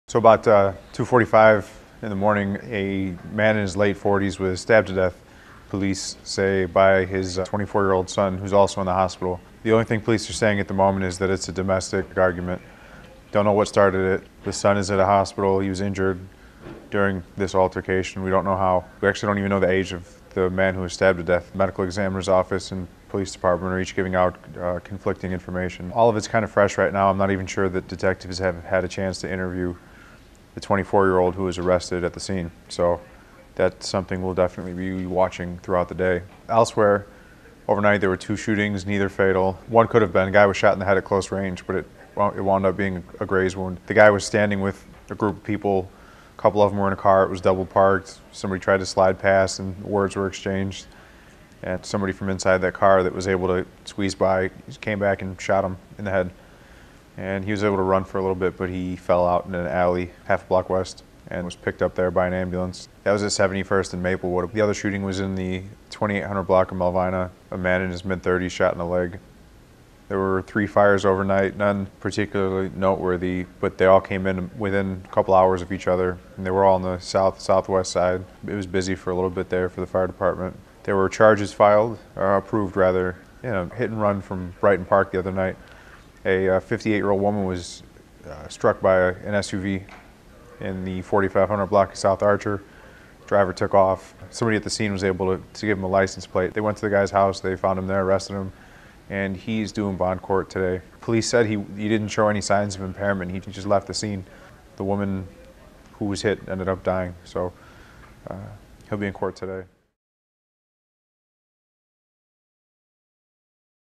Chicago overnight crime report (Sunday, March 24, 2013)
Chicago Tribune reporter Peter Nickeas with details on last night's crime in Chicago, including multiple Southside fires, a shooting in McKinley Park, and a stabbing in the Kennedy Park neighbordhood.